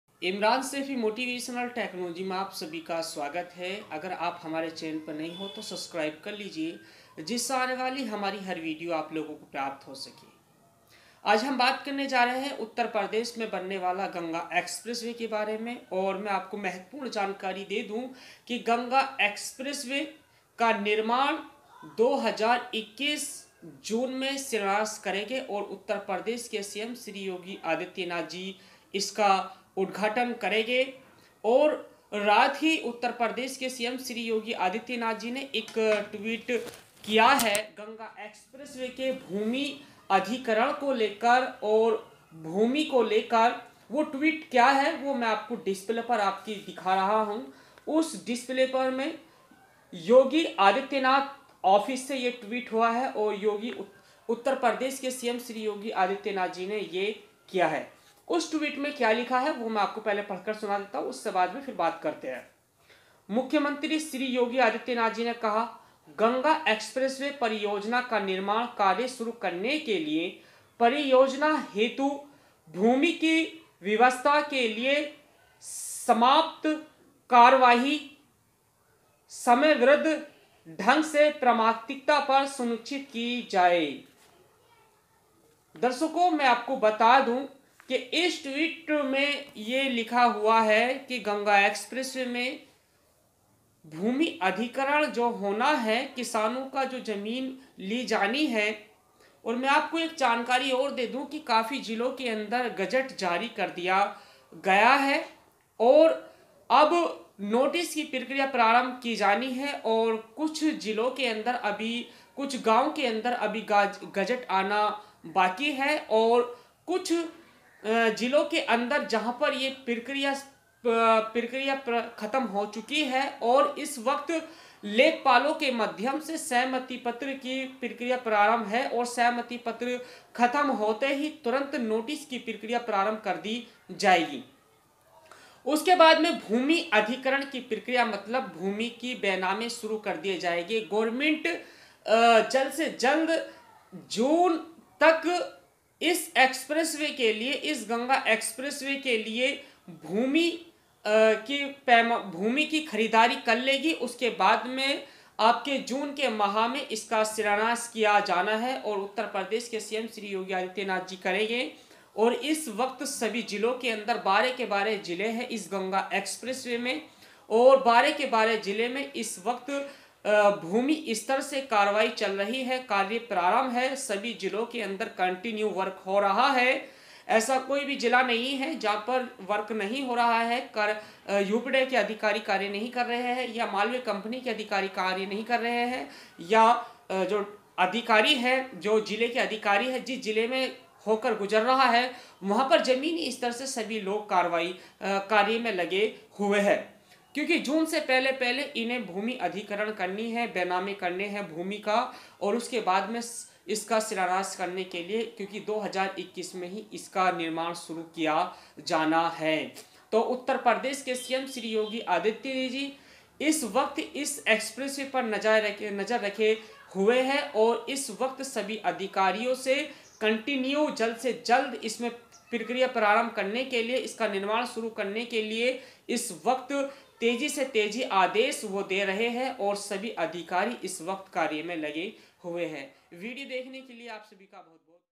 गंगा एक्सप्रेसवे में भूमि अधिग्रहण को योगी आदित्यनाथ ने दी मंजूरी।।Ganga Expressway।।Up Expressway
Ganga Expreesway Cm Yogi Adityanath Twitte ganga Expreesway bhumiadhikran...